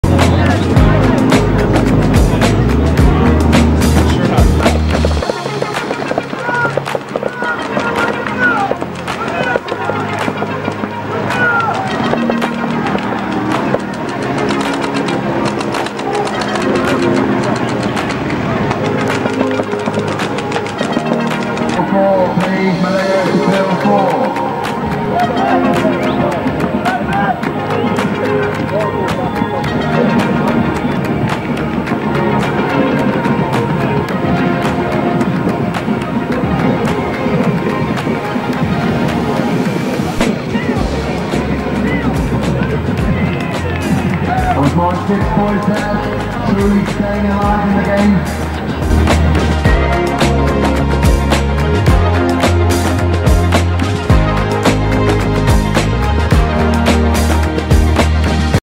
🇬🇧 BEE GEES: Is This MAURICE GIBB's Final Public Appearance at an Event? 👑🐀 🔫🏟️🏅🏆
BEE GEES: MAURICE GIBB (Commander Mo, Team: Royal Rat Rangers 👑 🐀) His hair appears to grow longer during the paintball tournament!  Venue: National Sports Centre 🏟️ Crystal Palace, UK. Campaign Cup, 13-15 September 2002. I think this might have been Maurice's last public appearance at an event in Europe...